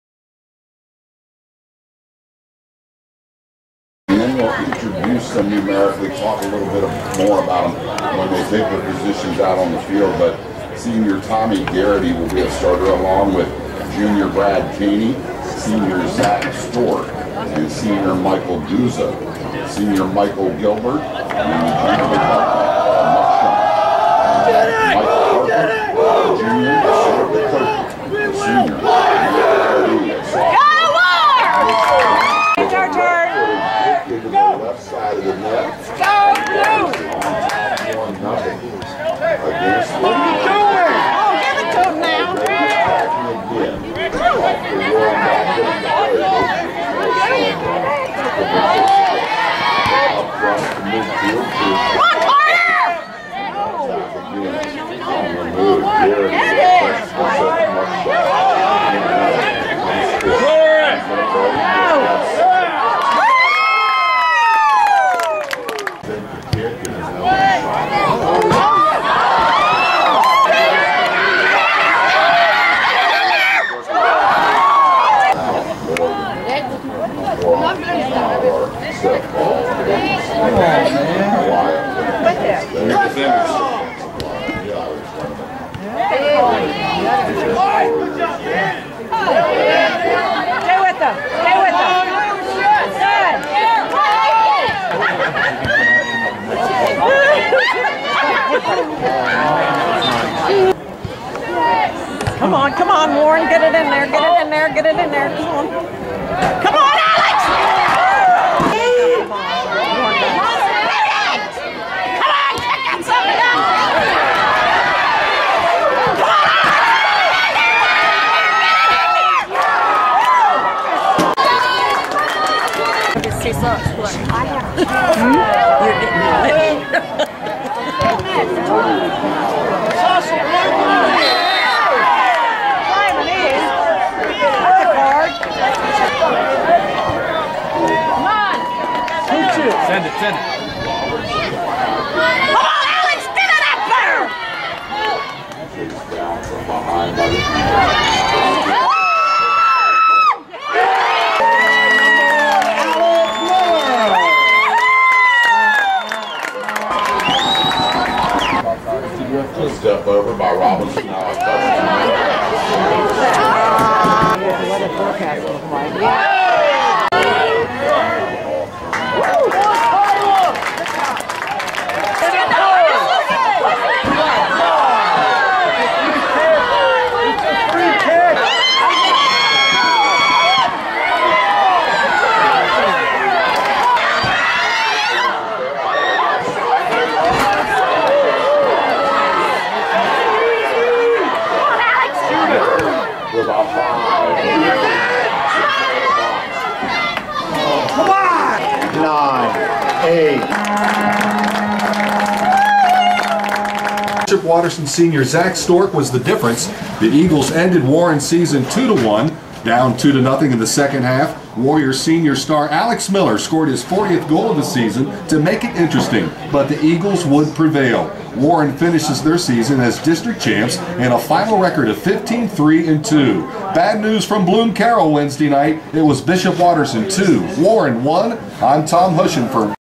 Warren Boys make it to Regional Tournament - Fall to Bishop Watterson 2-1
Congratulations to Warren Boys for a great season!  The regional soccer tournament playoff game was strong on both sides, but Bishop Watterson came out ahead.  It was a close game, and a few inches on several shots on goal could have easily changed the scoreboard.  Here are the highlights - enjoy!